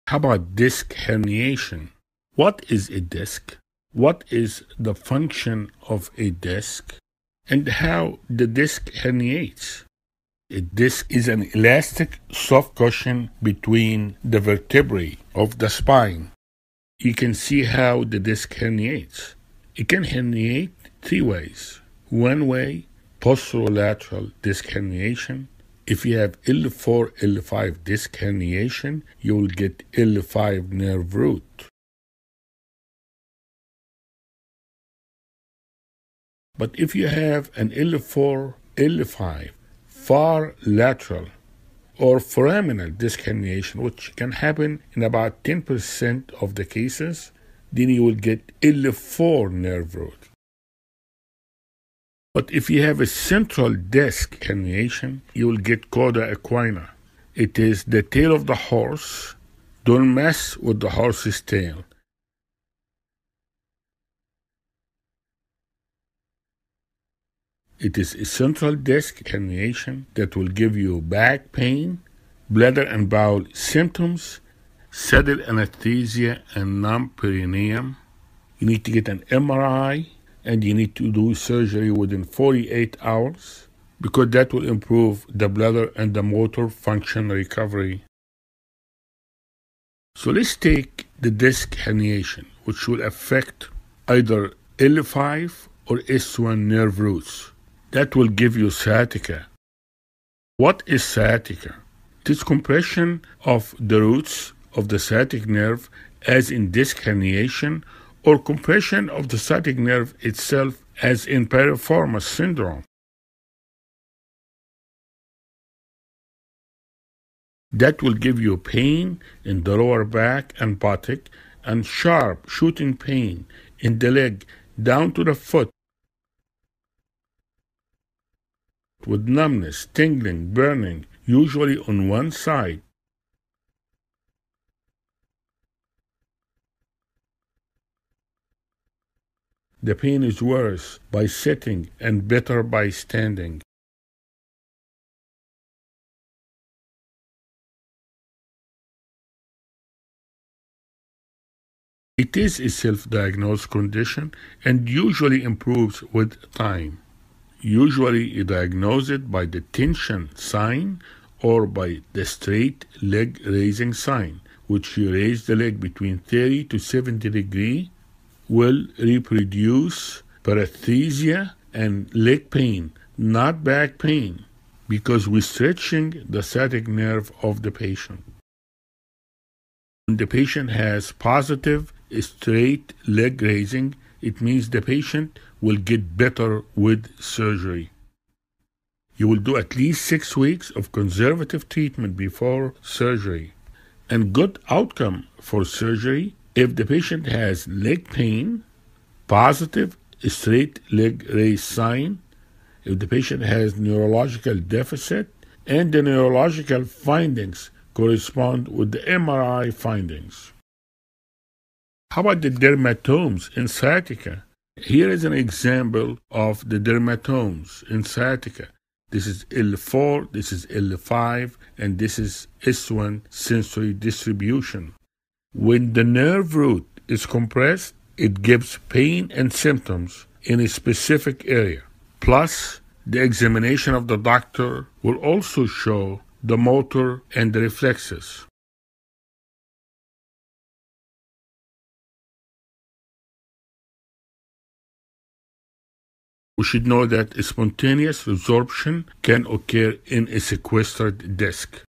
Sciatica And Lumbar Disc Herniation Causes Symptoms Diagnosis And Surgery
How about a disc herniation? What is a disc? What is the function of a disc, and how does the disc herniate? A disc is an elastic, soft cushion between the vertebrae of the spine. You can see how the disc herniates. It can herniate in three ways. One way is a posterolateral disc herniation. If you have an L4–L5 disc herniation, it will affect the L5 nerve root. But if you have an L4–L5 far-lateral or foraminal disc herniation, which can happen in about 10% of cases, then you will affect the L4 nerve root. But if you have a central disc herniation, you will get cauda equina. It is the tail of the horse. Do not mess with the horse’s tail. It is a central disc herniation that will give you back pain, bladder and bowel symptoms, saddle anesthesia, and a numb perineum. You need to get an MRI, and you need to do surgery within 48 hours, because that will improve bladder and motor function recovery. So let’s take the disc herniation, which will affect either the L5 or S1 nerve roots. That will give you sciatica. What is sciatica? It is compression of the roots of the sciatic nerve, as in disc herniation, or compression of the sciatic nerve itself, as in piriformis syndrome. That will give you pain in the lower back and buttock, and sharp, shooting pain in the leg down to the foot with numbness, tingling, and burning, usually on one side. The pain is worse with sitting and better with standing. It is a self-diagnosed condition and usually improves with time. Usually, you diagnose it by the tension sign or by the straight-leg-raising sign, which, if you raise the leg between 30 to 70 degrees, will reproduce paresthesia and leg pain, not back pain, because we are stretching the sciatic nerve of the patient. When the patient has a positive straight-leg-raising test, it means the patient will get better with surgery. You will do at least six weeks of conservative treatment before surgery, and there is a good outcome for surgery if the patient has leg pain, a positive straight-leg-raise sign, if the patient has a neurological deficit, and the neurological findings correspond with the MRI findings. How about the dermatomes in sciatica? Here is an example of the dermatomes in sciatica. This is L4, this is L5, and this is the S1 sensory distribution. When the nerve root is compressed, it gives pain and symptoms in a specific area, and the examination by the doctor will also show the motor and the reflexes. We should know that spontaneous resorption can occur in a sequestered disc
Quizzes

1) Which nerve root is affected by an L4–L5 posterolateral disc herniation?
A. L3
B. L5
C. S1
D. L4
Answer: B. L5
Explanation: Posterolateral herniation at L4–L5 compresses the traversing L5 root.

2) An L4–L5 far-lateral (foraminal) disc herniation compresses which nerve root?
A. L4
B. L5
C. S1
D. L3
Answer: A. L4
Explanation: Far-lateral herniations compress the exiting root.

3) Which finding indicates emergency surgery within 48 hours?
A. Isolated buttock pain
B. Bladder/bowel dysfunction with saddle anesthesia
C. Pain relieved by sitting
D. Night pain only
Answer: B. Bladder/bowel dysfunction with saddle anesthesia
Explanation: These are red flags for cauda equina syndrome.

4) Straight-leg raise is positive in which range?
A. 0–20°
B. 30–70°
C. 80–100°
D. Beyond 110°
Answer: B. 30–70°
Explanation: This range stretches the sciatic nerve.

5) A true positive straight-leg raise reproduces:
A. Back pain only
B. Leg pain and paresthesia, not isolated back pain
C. Hip groin pain
D. Knee swelling
Answer: B. Leg pain and paresthesia, not isolated back pain
Explanation: SLR should cause radicular leg pain.

6) Good predictors for surgery outcome include:
A. Leg pain, positive SLR, neurological deficit, and MRI concordance
B. Back pain alone
C. Negative SLR
D. Symptoms only at night
Answer: A. Leg pain, positive SLR, neurological deficit, and MRI concordance
Explanation: Symptom–sign–imaging alignment predicts success.

7) L5 sensory symptoms occur over:
A. Medial calf
B. Dorsum of foot and great toe
C. Heel
D. Posterior thigh
Answer: B. Dorsum of foot and great toe
Explanation: L5 involves dorsum of foot and hallux.

8) Which reflex is decreased in S1 radiculopathy?
A. Patellar
B. Biceps
C. Achilles
D. Supinator
Answer: C. Achilles
Explanation: The Achilles tendon reflex is mediated by S1.

9) Pain worse with sitting and better with standing suggests:
A. Hip arthritis
B. Sacroiliitis
C. Lumbar disc herniation sciatica
D. Meralgia paresthetica
Answer: C. Lumbar disc herniation sciatica
Explanation: Flexion worsens disc pain, extension relieves it.

10) Sciatic nerve compression in piriformis syndrome causes:
A. Trochanteric bursitis
B. Sciatica-like pain
C. Plantar fasciitis
D. SI joint pain
Answer: B. Sciatica-like pain
Explanation: Piriformis entraps the sciatic nerve, mimicking radiculopathy.